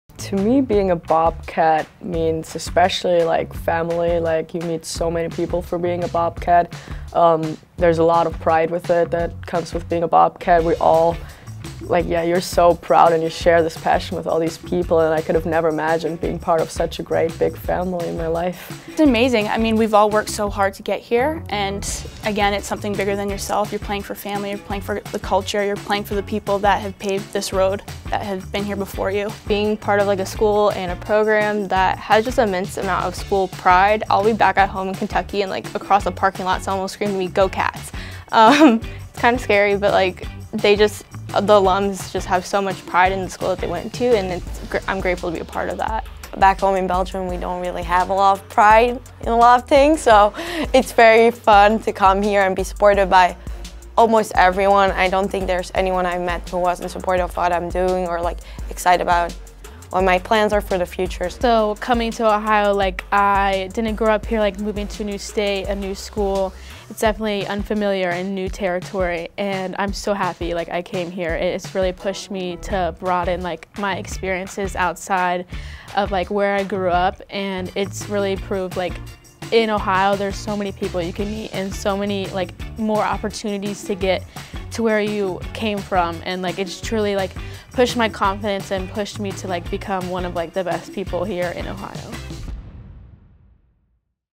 Ohio Field Hockey (2021-22): What It Means To Be A Bobcat
Ohio Field Hockey told us what it means to be a Bobcat